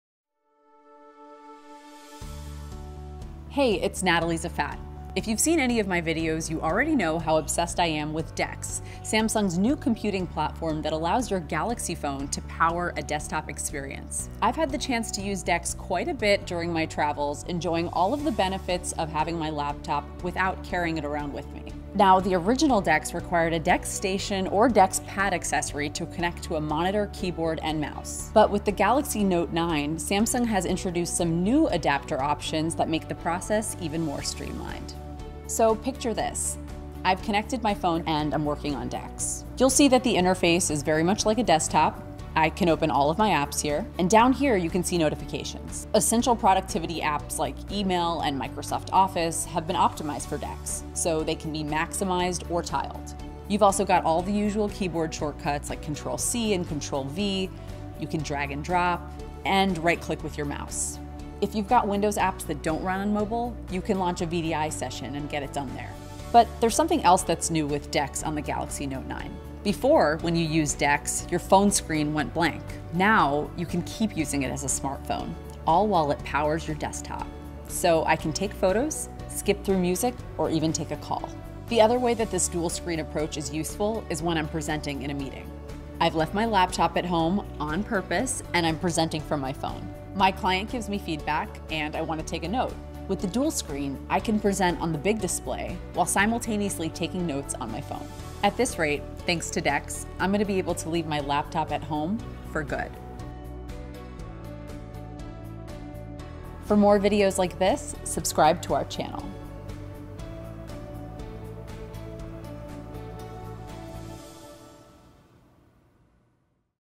Getting the Most Out of Samsung DeX with Dual Mode
Enjoy the benefits of a laptop – without having to lug one around. Samsung video correspondent Natalie Zfat discusses the latest advancements in Samsung DeX available on the new super powerful Galaxy Note9.

Learn more about how DeX can deliver a full desktop experience to your Galaxy phone